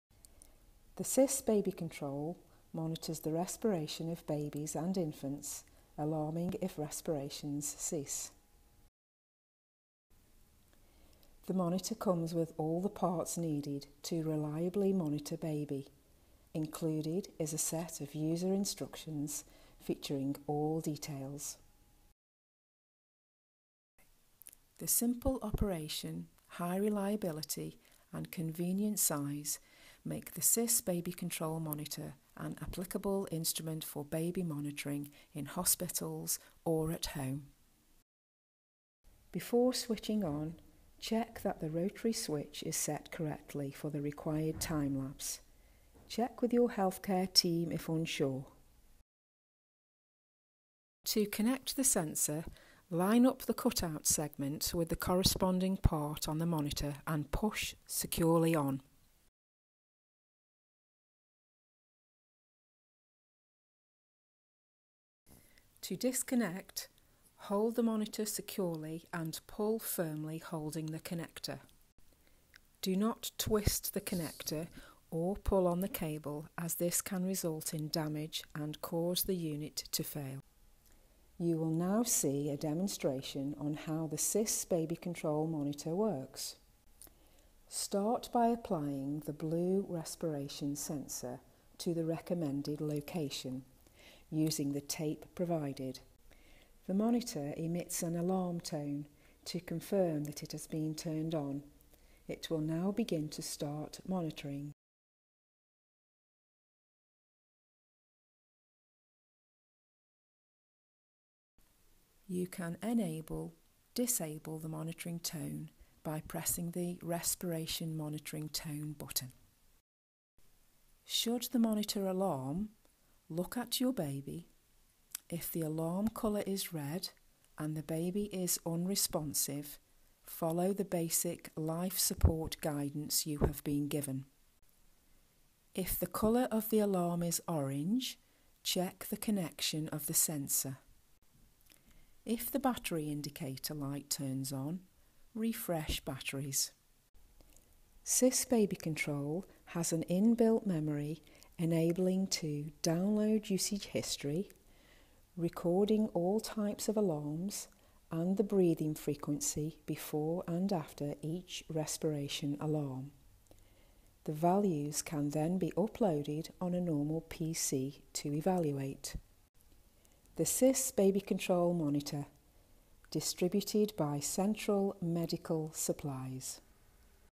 SISS Baby Control Apnoea Monitor
The SISS Babycontrol is an Apnoea monitor for newborns that monitors the respiratory function both when stationary and mobile. A reusable body sensor is placed on the child's abdominal wall and records the respiratory movements.

The SISS Babycontrol monitor notifies of threatening respiratory pauses with both an acoustic and visual alarm. The alarm priority can be ascertained with the colour-coded display.

Times, types and frequencies of alarms are stored in the integrated storage of the SISS Babycontrol and can then be transferred to a PC, where they can be read and stored.